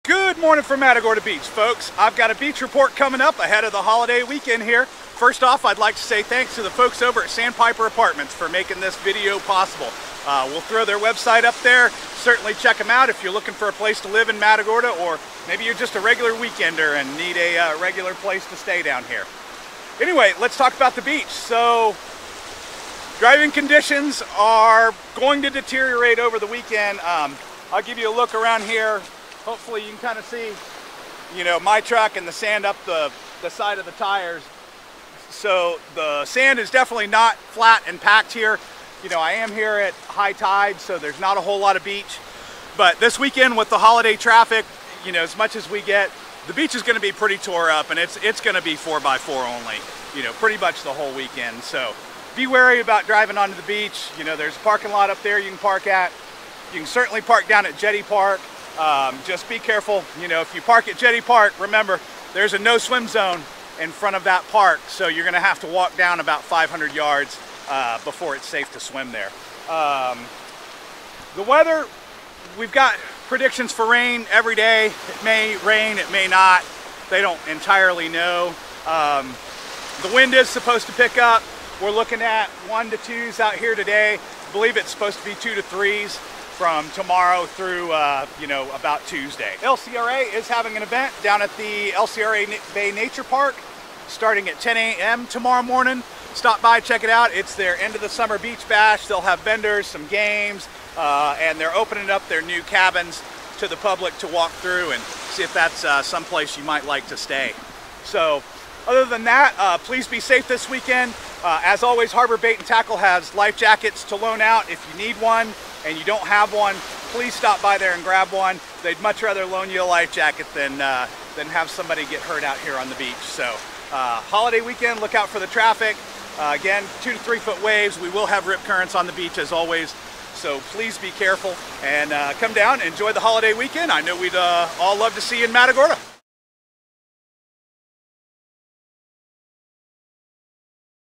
Labor Day Weekend 2022
Here is the holiday weekend beach report. Don't forget the mosquito spray this weekend!